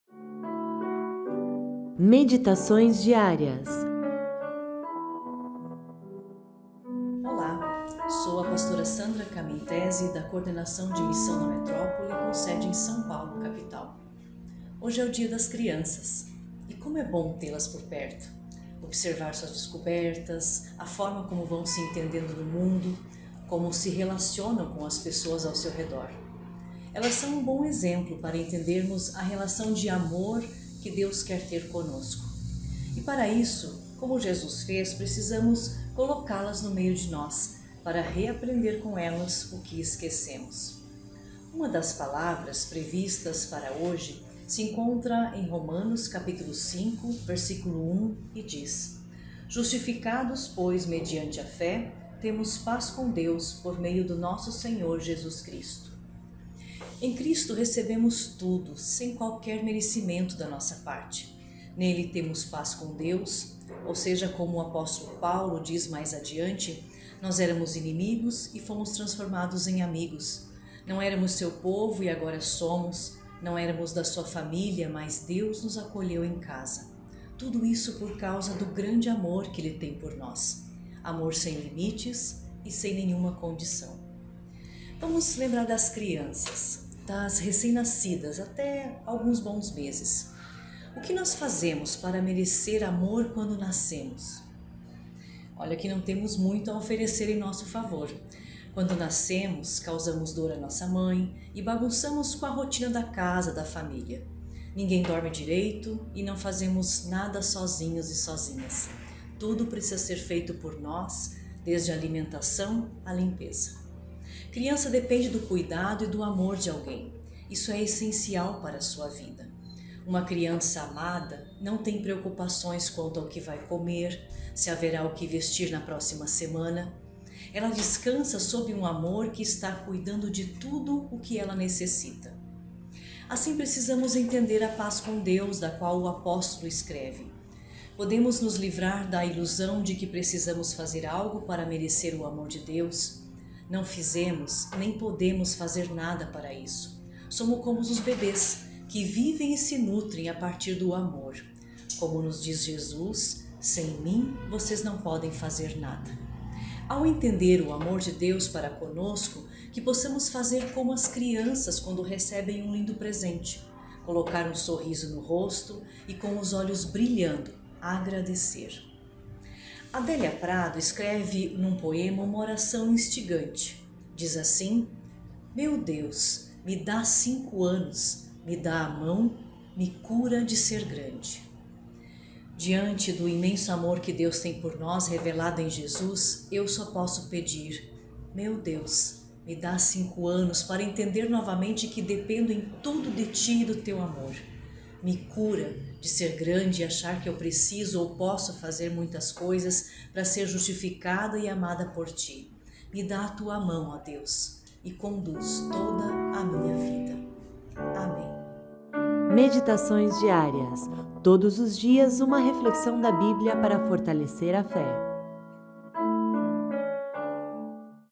MEDITAÇÕES DIÁRIAS: Salmo 130.4 e Lucas 23.34 - 11 de outubro | IECLB
Meditações Diárias: todos os dias, uma reflexão da Bíblia para fortalecer a fé 🙏

Acompanhe a reflexão para o dia 12 de outubro de 2024, sobre Levítico 10.3
Romanos 5.1, conduzida pela Pa. Sandra Kamien Tehzy, Coordenadora de Missão na Metrópole da IECLB, localizada em São Paulo, Sínodo Sudeste.